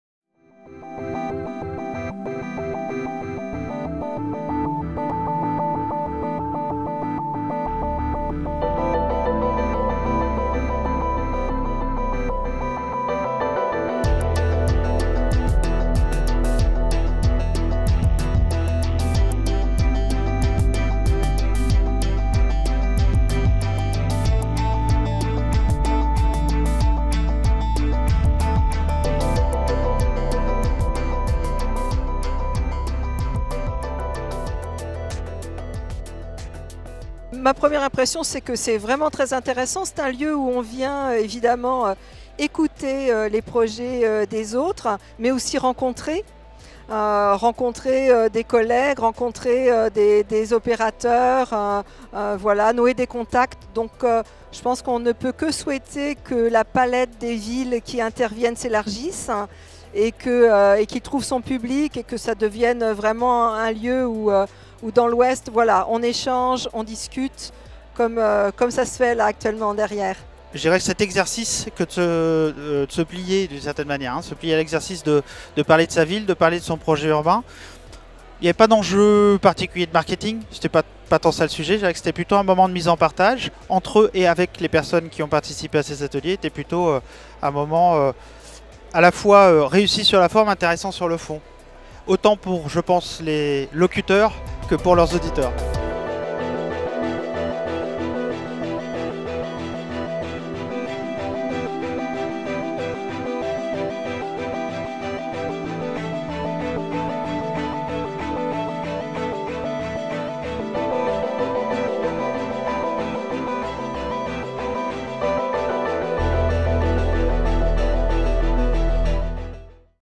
Forum des Projets Urbains Grand Ouest
Retour en images sur la première édition du Forum des Projets Urbains Grand Ouest, le 29 mars 2017 à la Cité des Congrès de Nantes.

avec 400 participants ;

17 ateliers projets : Alençon, Beaucouzé, Brest, Carquefou, Guérande, La Baule, La Rochelle, La Roche-Sur-Yon, Lorient, Nantes, Pontchâteau, Rennes, Saint-Herblain, Saint-Nazaire, Savenay... ;

Une séance d’ouverture : "Nantes Saint-Nazaire : la métropole en
projets depuis les rives de la Loire" en présence de Johanna Rolland, maire de Nantes et présidente de Nantes Métropole, David Samzun, maire de Saint-Nazaire et Guillaume Hébert, Une Fabrique de la ville ;

Un atelier stratégique : "Bilbao : l’innovation toujours en
marche ?" en présence de Pablo Otaola, Directeur général de
Zorrotzaurre et Ariella Masbourgi, Grand prix de l’urbanisme 2016 ;

Une séance plénière : "Quelle(s) stratégie(s) pour les villes portuaires ?" en présence de Paul Tourret, directeur, Institut Supérieur d’Economie Maritime (ISEMAR), Franck Mousset, directeur Territoires, accès, environnement, Grand Port Maritime Nantes Saint-Nazaire et Claire Guihéneuf, directrice, Brest Aménagement Métropole.